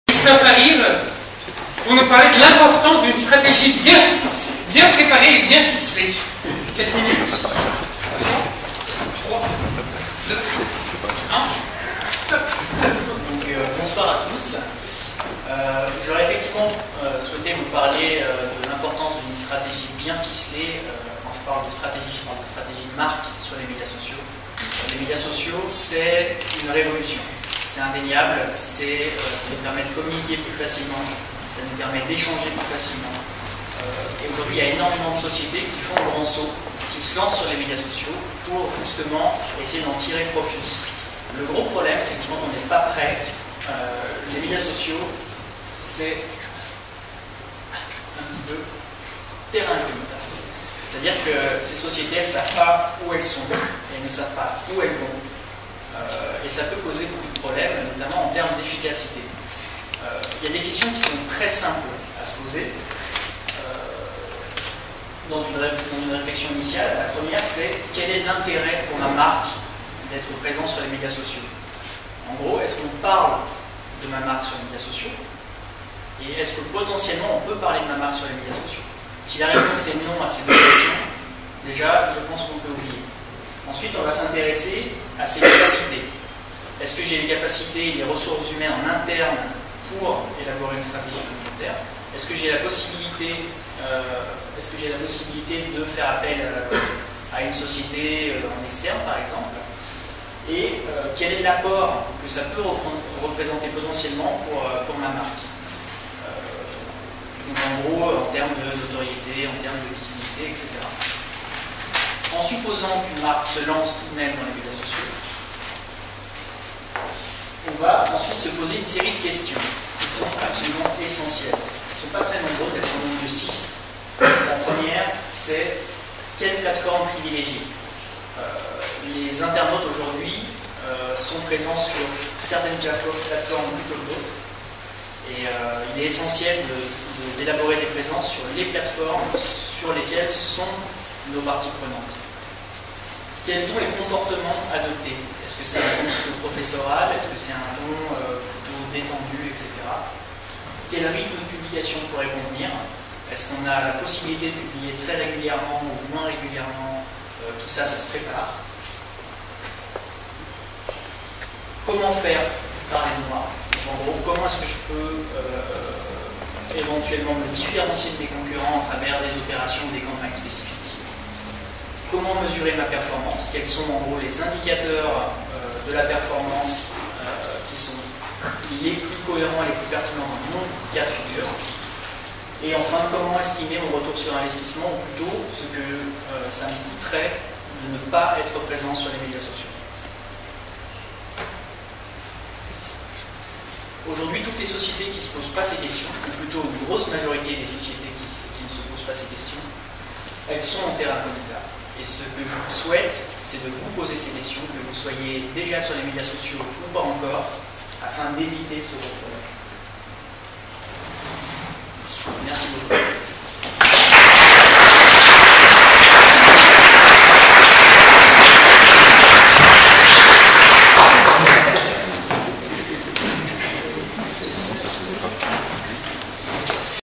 De l'importance d'une stratégie bien préparée et bien ficelée - Christophe Ramel
Conférence éclair ("lightning talk") présentée dans le cadre de la conférence "Comprendre Internet et les réseaux sociaux" organisée par le blog InternetEtMoi.fr (Sébastien Desbenoit) le 21 février 2012 à la mairie de Lyon 7e.

Cet évènement fait partie des "off" autour de la conférence www2012 (16 au 20 avril 2012 - Lyon).